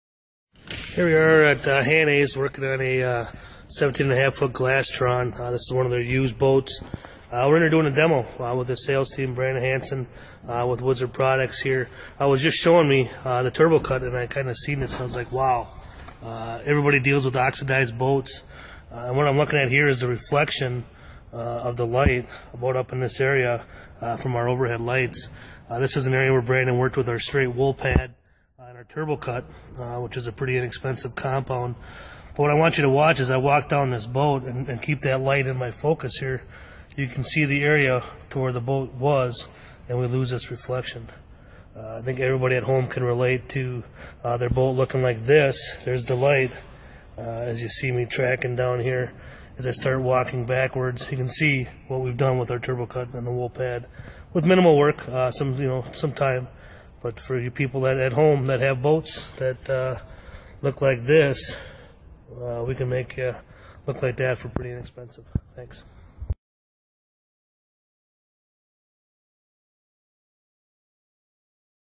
Wizards Turbo Cut Compound Boat Demo
Before and after reflection in fiberglass/gelcoat boat using Wizards Turbo Cut Compound and a wool buffing pad.